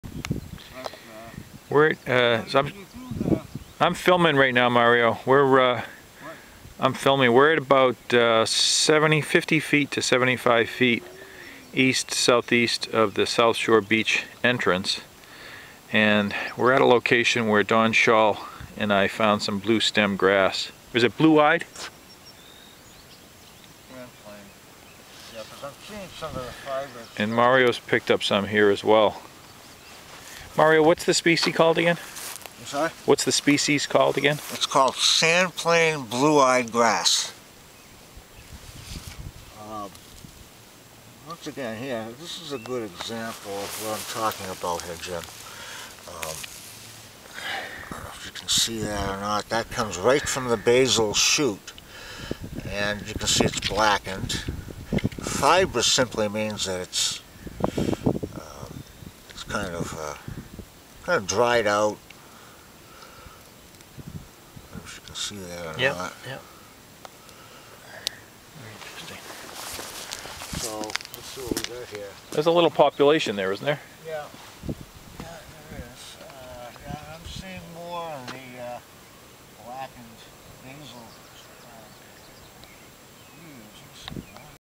Blue eyed grass survey by Mario DiGregorio, Moshup Trail, Aquinnah, MA; June 16, 2009
Endangered species review, Moshup Trail, Aquinnah